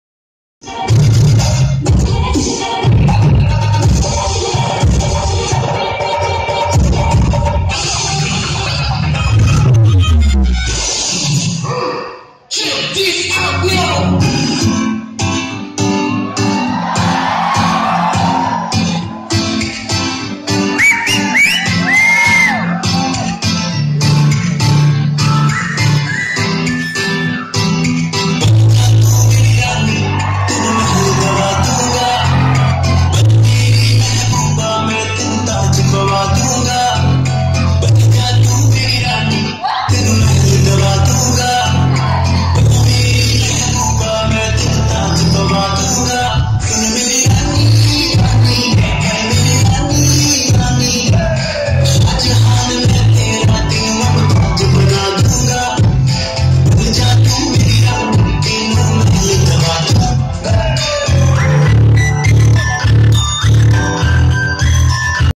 Hip hop forms
Hip hop dance is a part of dance world which is basically perform on instrumental music. Now a days remix version also available. This video is showing the remix version of hip hop which is wonderfully performed by the participants on stage. You can see the video in live and enjoy!!